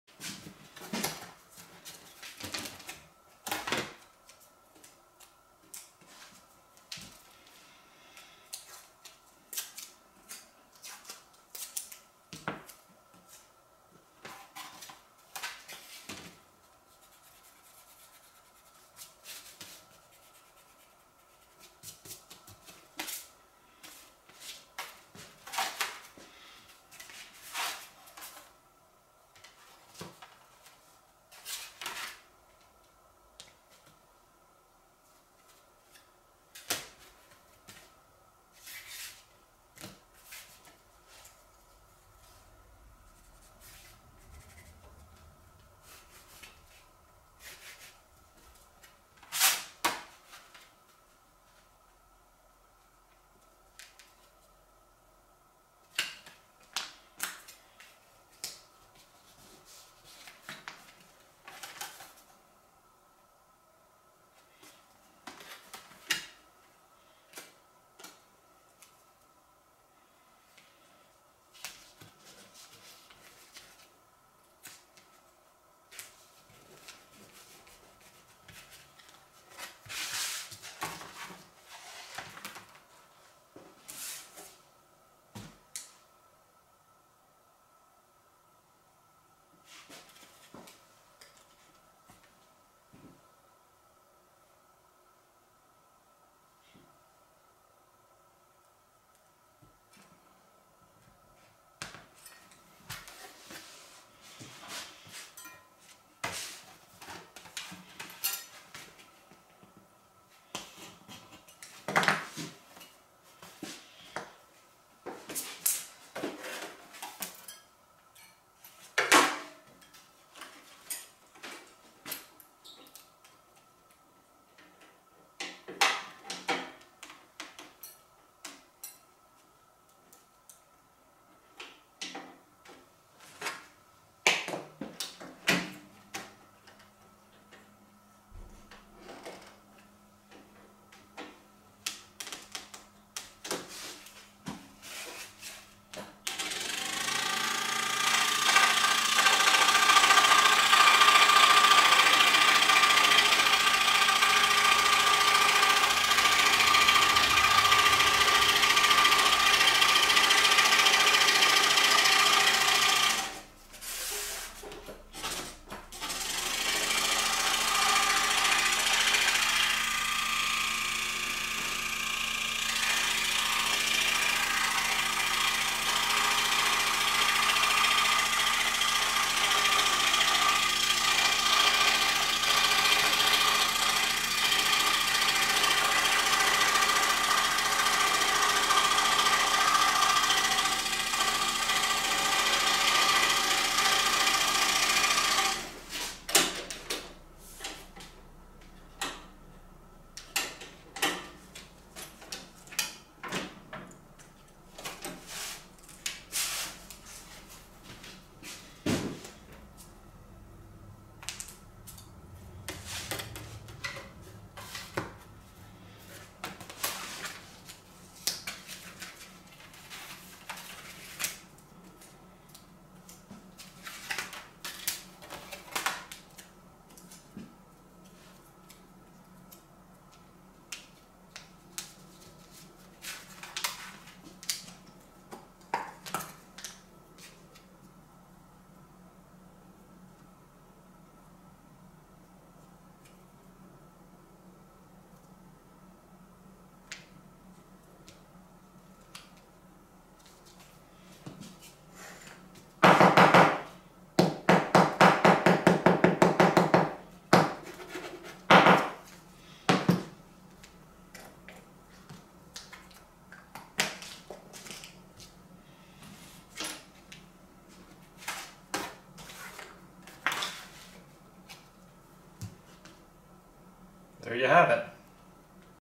Marquetry Process
One leaf at a time. The background is made up of segments so I don't have to work with the entire sheet at once. Each leaf takes 5-10 minutes to set up and cut.